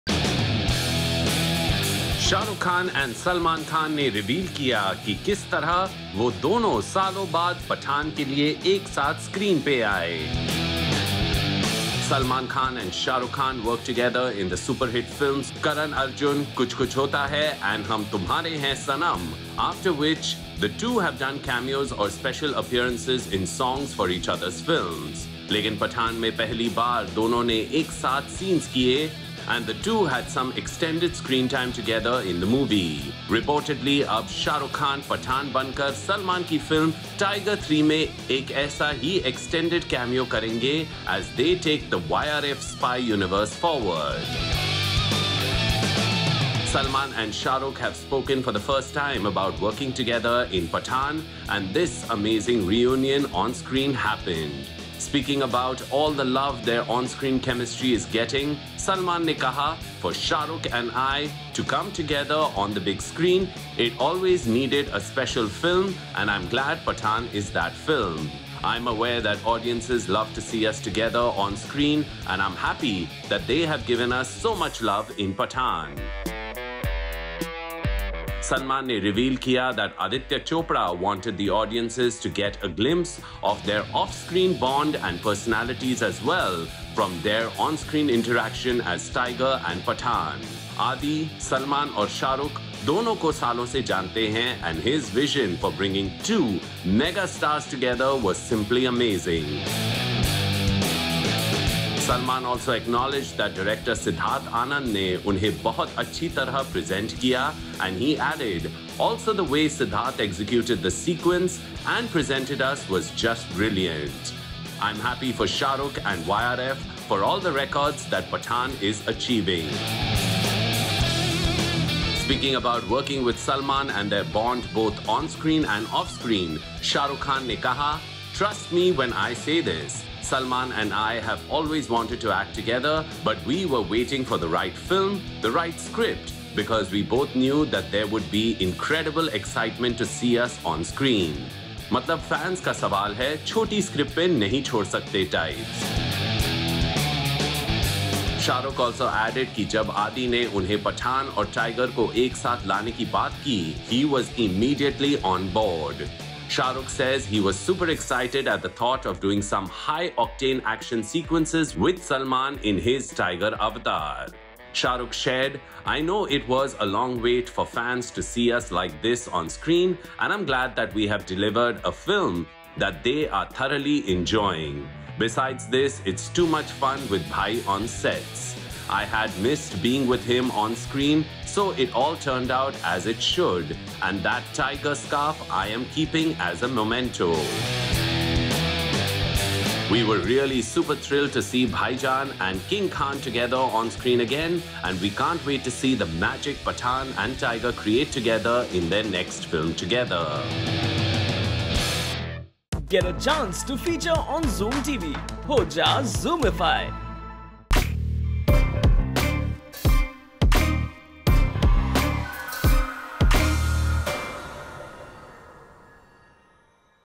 Salman Khan & Shah Rukh Khan spill the beans about their REUNION in Pathaan
Salman Khan and Shah Rukh Khan are two most well-known actors of Bollywood. Both have worked together for many films such as Karan Arjun, Kuch Kuch Hota Hai, and Hum Tumhare Hain Sanam. Salman Khan praised Siddharth Anand for the way he presented him and Shah Rukh Khan on the screen in Pathaan. According to the sources, Shah Rukh Khan will be playing a role in Salman Khan's next 'Tiger 3'. Check out the video for more!